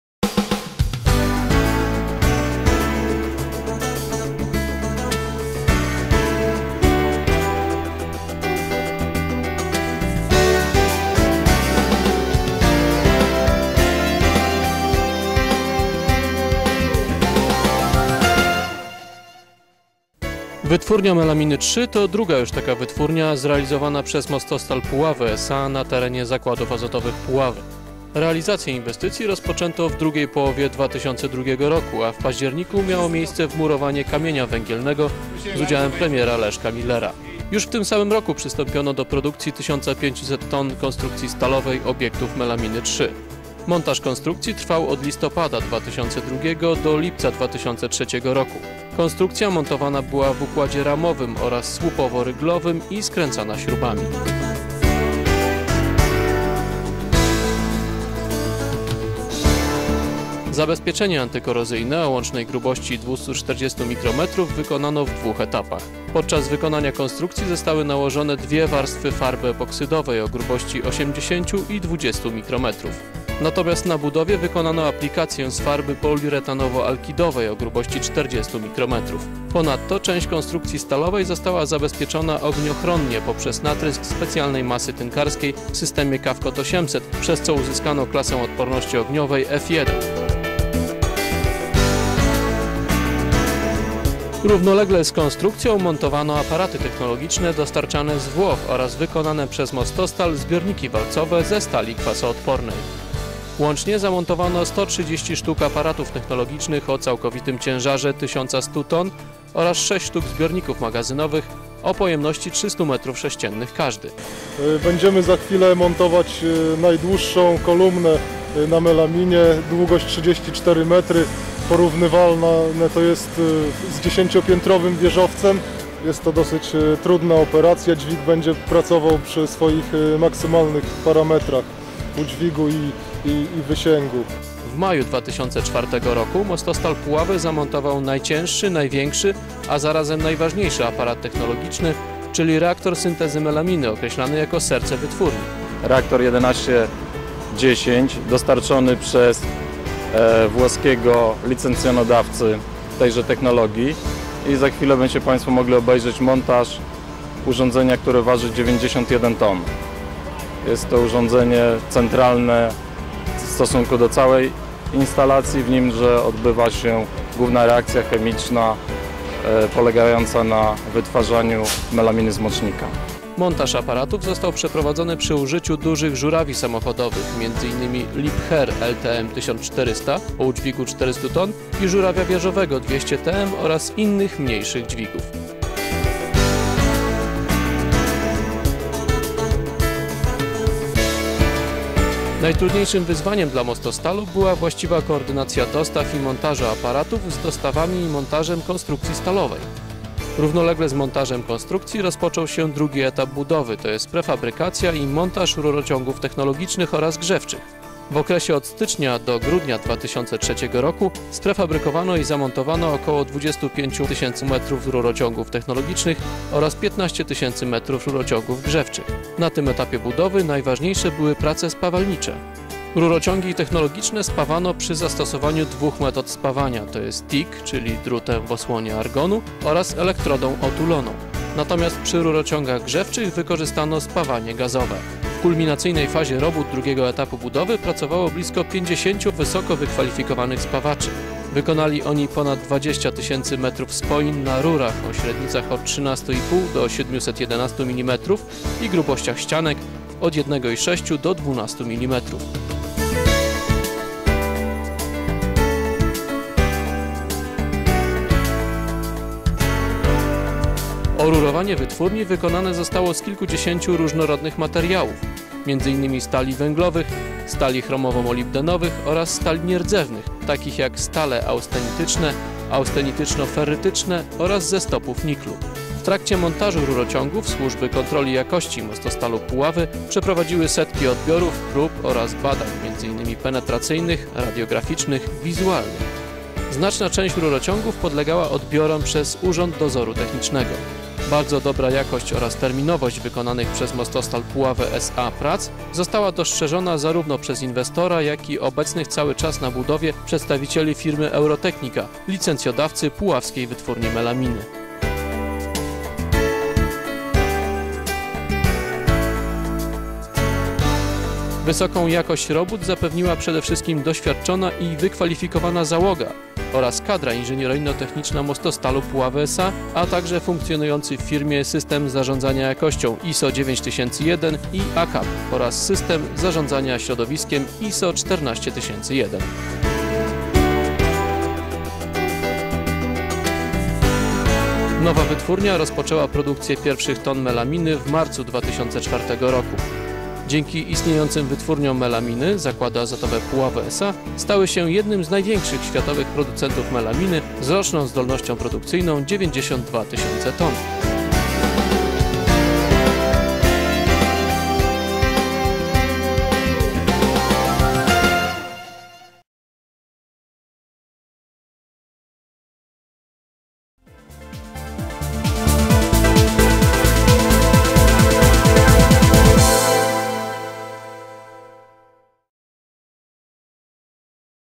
Wytwórnia Melaminy III w Zakładach Azotowych "Puławy" S.A.
Film o budowie Melamina III w Zakładach Azotowych "Puławy" S.A.